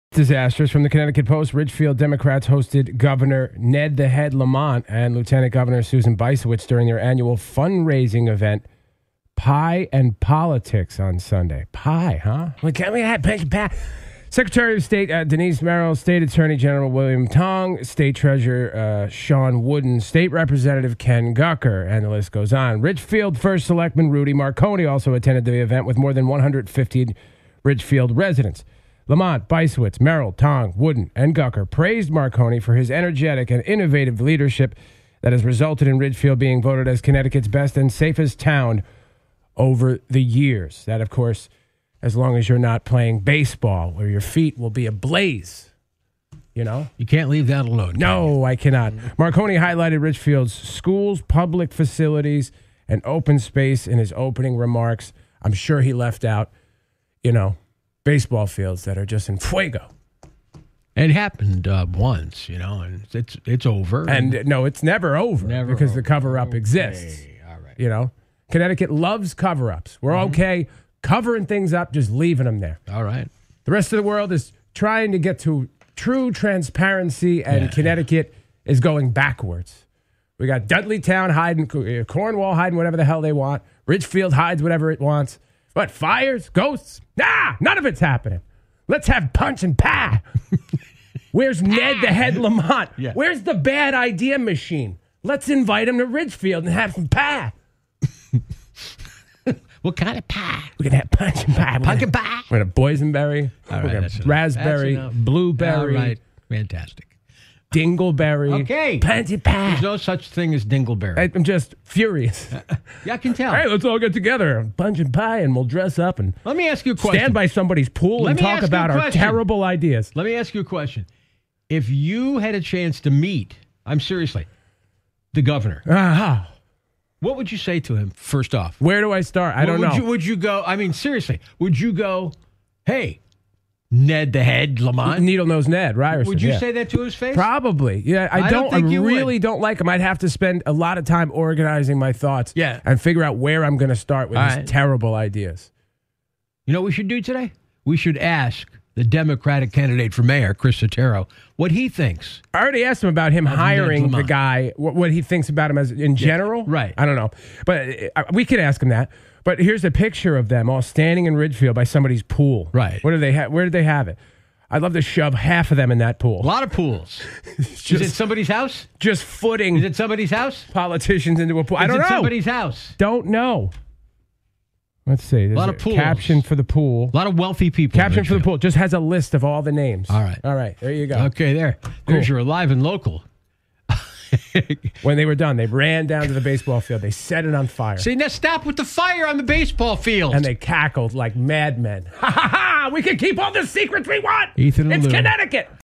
CT Gov. Ned Lamont Was in Ridgefield, CT For Pie
Was it punch and pie? Was Ned here to celebrate the keeping of secrets or pat Ridgefield on the back for covering up a fire? So many questions, so little time.